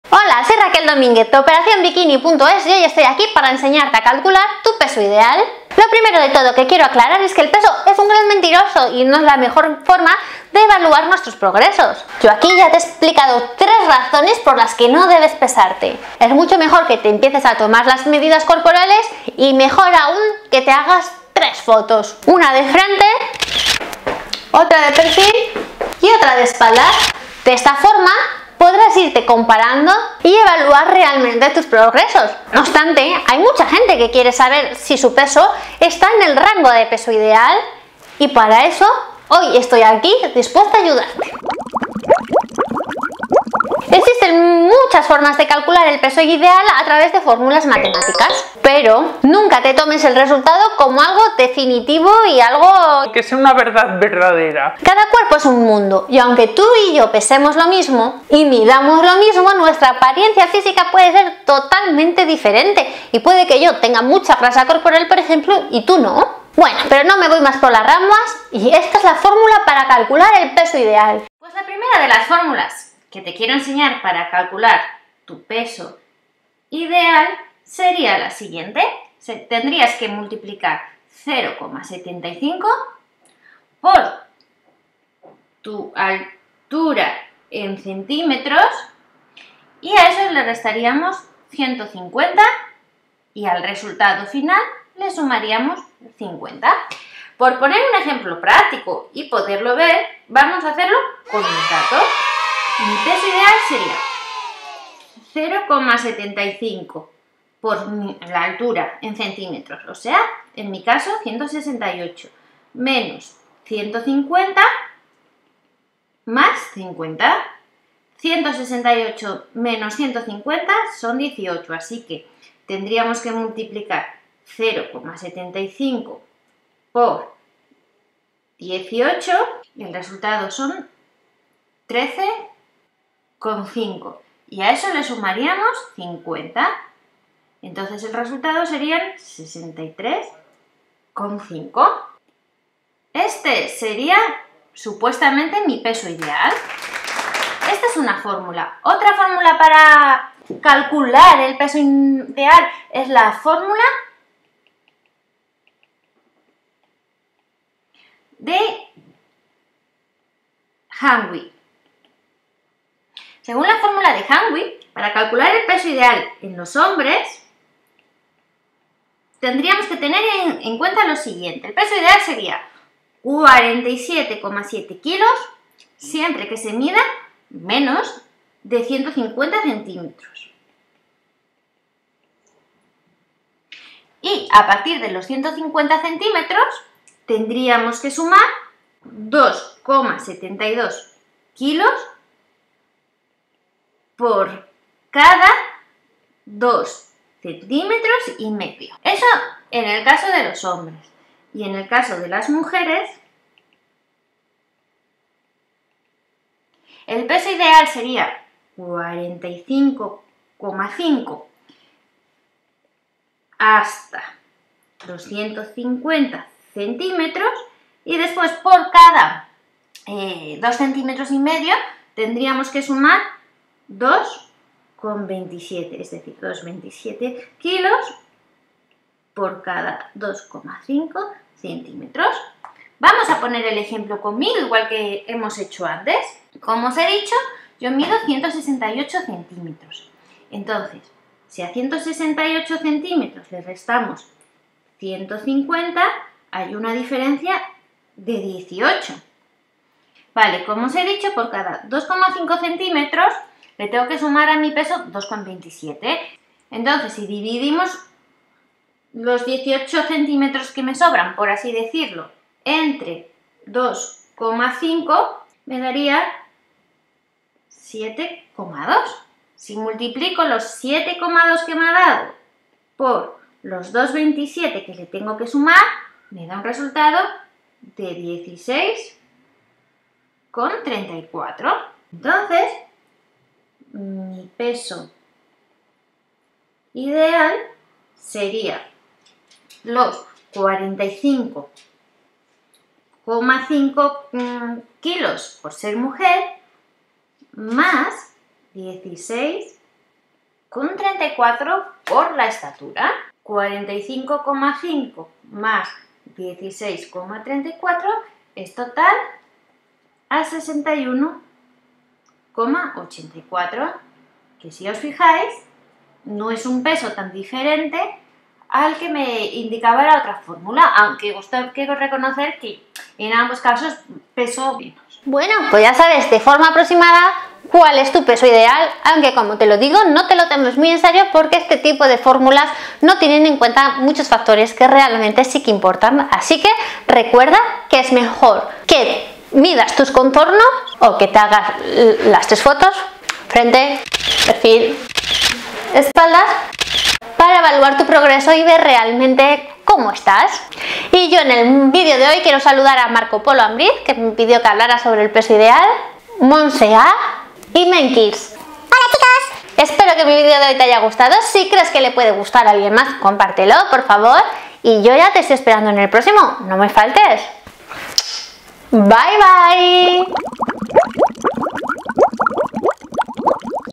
Aprende a calcular peso ideal ⭐⭐⭐⭐⭐
Lo primero que te quiero aclarar que el peso es un gran mentiroso y no es la mejor manera de evaluar nuestro estado de forma. Es mucho mejor que te tomes las medidas o te hagas 3 fotos. Una de espalda, otra de frente y otra de perfil.

Aunque dos personas pesen y midan lo mismo su apariencia física puede ser muy diferente y una de ellas tener mucha grasa corporal y la otra no.

No obstante te dejo dos fórmulas para que aprendas a calcular tu peso ideal:

Fórmula 1:
Peso ideal= 0,75x(altura en cm -150)+50

Fórmula Hamwi:

Peso ideal en hombres: el peso ideal para los hombres es de 47,7 kilos para los primeros 150 cm de altura y se incrementaría en 2,72 kilos por cada 2,5 centímetros a partir de los 150.

Peso ideal en mujeres: el peso ideal para las mujeres es de 45,5 kilos para los primeros 150 cm de altura y se incrementaría 2,27 kilos por cada 2,5 cm a partir de los 150 cm.